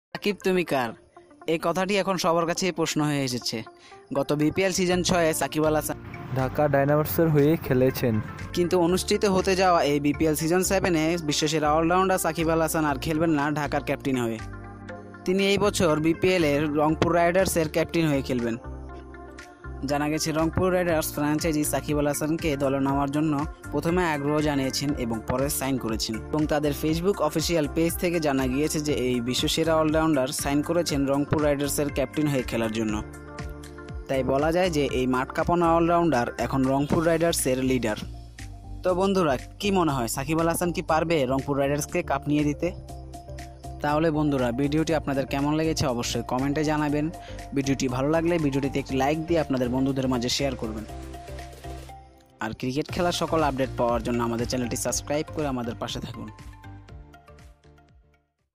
In which team Sakib will play? BPL 2019-20।। সাকিব তুমি কার???
Sakib তুমি কার? এই কথাটা এখন সবার কাছে  প্রশ্ন হয়ে এসেছে। গত বিপিএল সিজনে 6 সাকিব আল হাসান হয়ে খেলেছেন ঢাকা ডাইনামাইটস এর। কিন্তু অনুষ্ঠিত হতে যাওয়া এই বিপিএল সিজন 7 এ বিশ্ব সেরা অলরাউন্ডার সাকিব আল হাসান খেলবেন না ঢাকার ক্যাপ্টেন হয়ে। তিনি এই বছরের বিপিএল এ রংপুর রাইডারস এর ক্যাপ্টেন হয়ে খেলবেন। জানা গেছে রংপুর রাইডারস ফ্রাঞ্চাইজি সাকিব আল হাসান কে দলে নেওয়ার জন্য প্রথমে আগ্রহ জানিয়েছেন এবং পরে সাইন করেছেন। এবং তাদের ফেসবুক অফিসিয়াল পেইজ থেকে জানা গিয়েছে যে এই বিশ্ব সেরা অলরাউন্ডার ও সাইন করেছেন রংপুর রাইডারস এর ক্যাপ্টেন হয়ে খেলার জন্য। তাই বলা যায় যে এই মাঠ কাপানো অলরাউন্ডার এখন রংপুর রাইডারস এর লিডার।

বন্ধুরা ভিডিও টা আপনাদের কাছে কেমন লাগল? যদি ভালো লাগে অবশ্যই ভিডিও টি তে একটা লাইক দিবেন এবং ভিডিও টি শিয়ার করিয়েন আপনার বন্ধু এবং আত্মিয়দের সাথে।